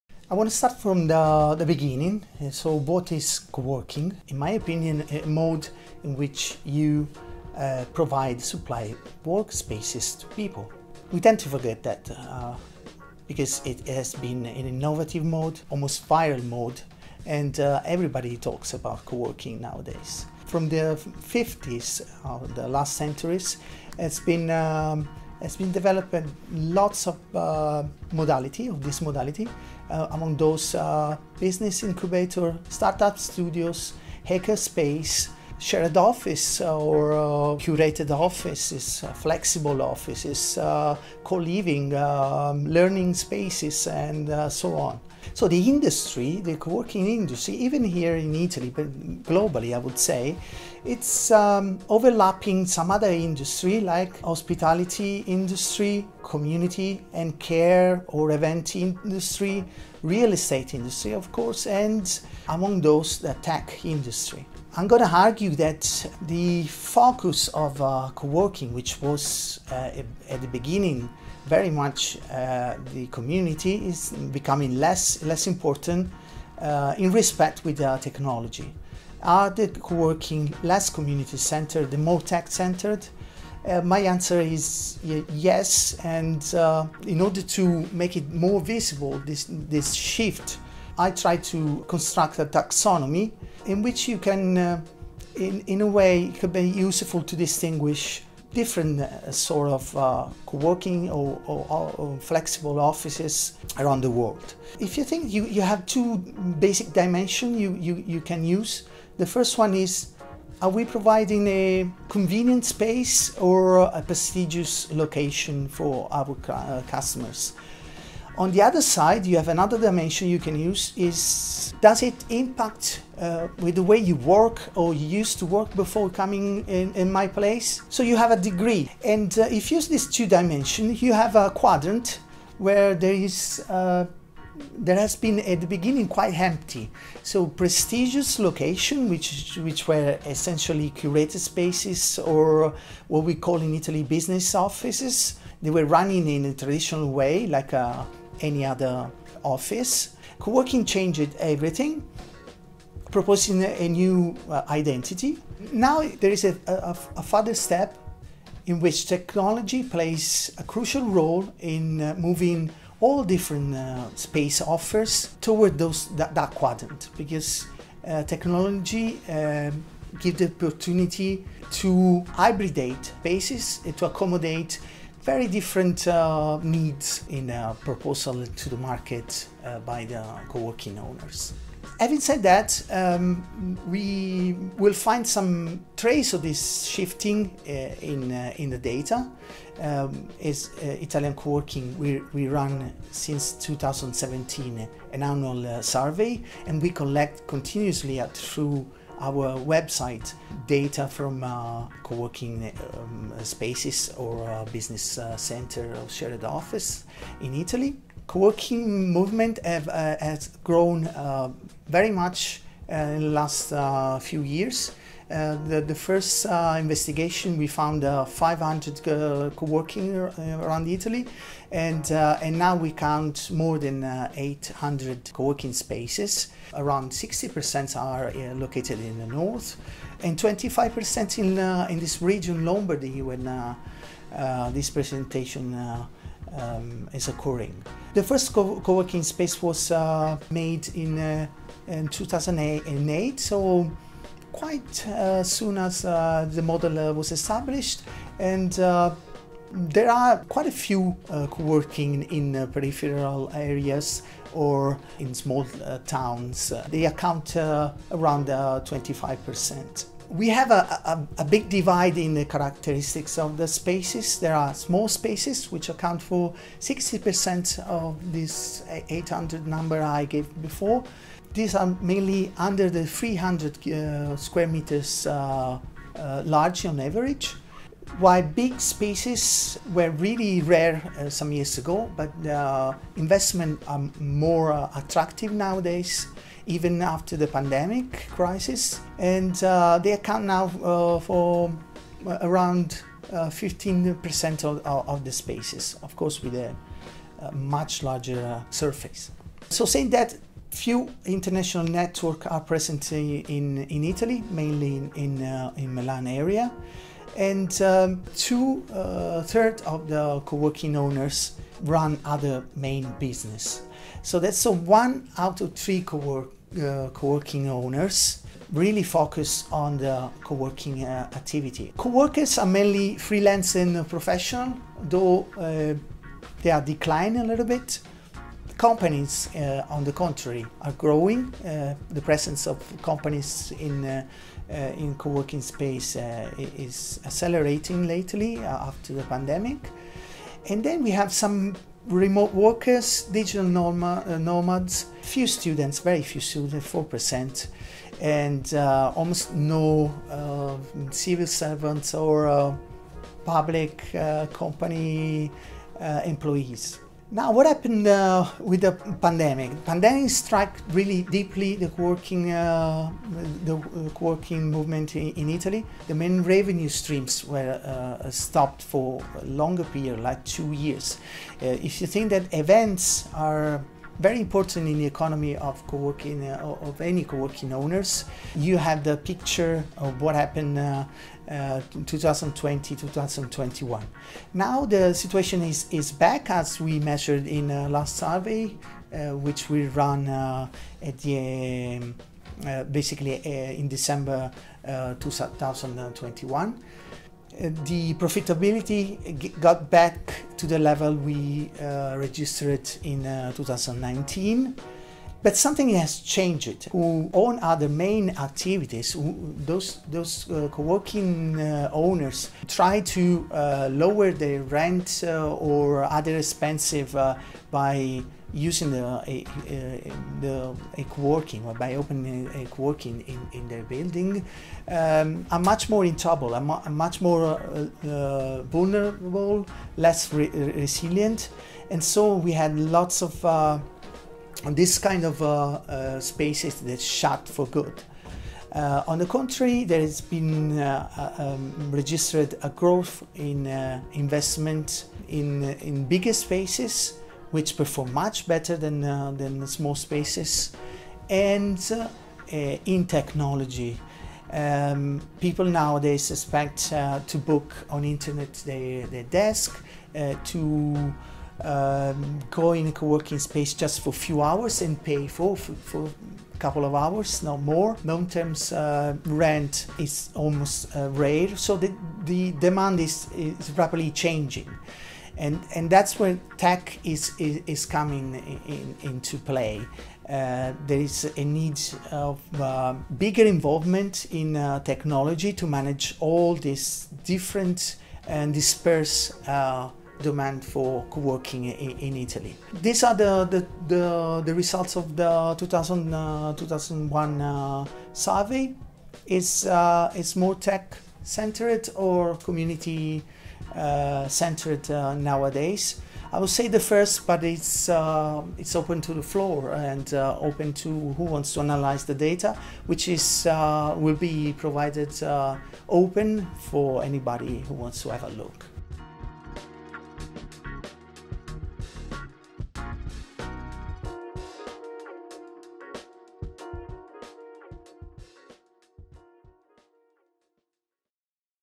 Coworking and Entrepreneurship - Michele Lo Russo.
Coworking and Entrepreneurship by Michele Lo Russo.
V Edition Summer School on Workplace Management 2022
Co-organized by Cost Action 18214.

The geography of New Working Spaces and impact on the periphery .